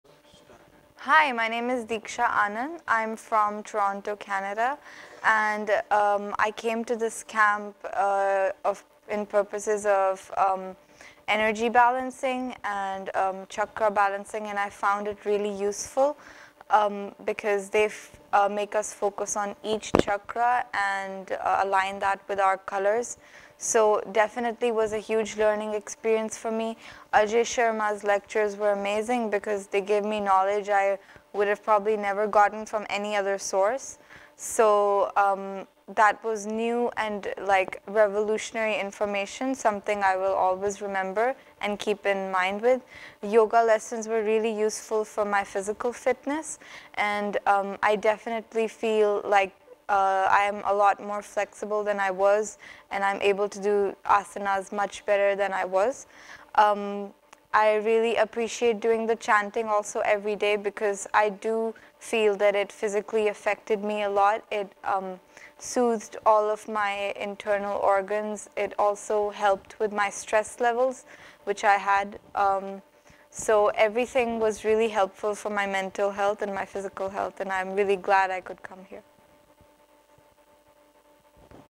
EnhanceMental &Physical Health by meditation
The Chakra And Colour Meditation Was Very Useful In Enhancing Her Mental Health. Yoga Sessions Made Her Physically Healthy. It Was  An Incredible Experience.